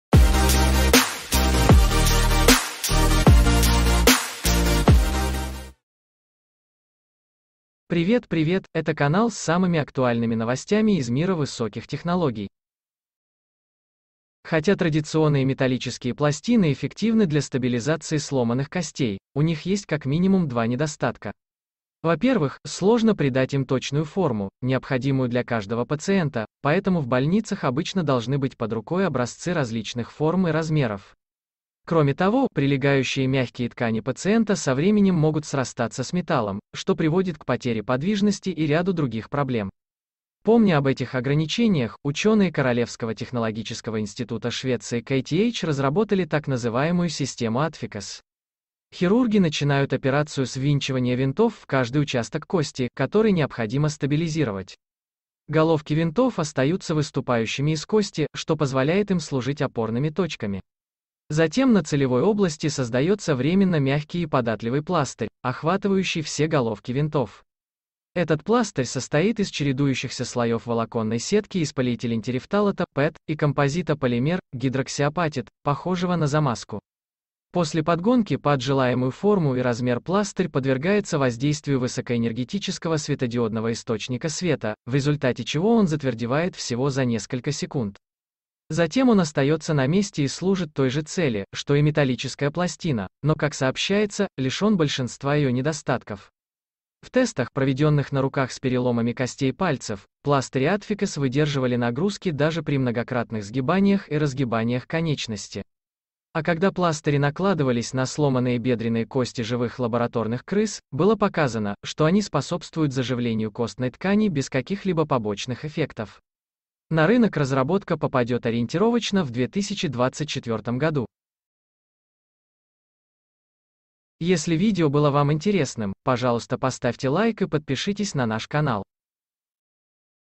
Пластырь для сращивания костей: быстро и надежно
В настоящее время хирурги-ортопеды используют металлические пластины на винтах, чтобы скрепить нестабильные сломанные кости. Однако вскоре может появиться менее проблемная, более гибкая альтернатива, основанная на светоотверждаемом композитном материале.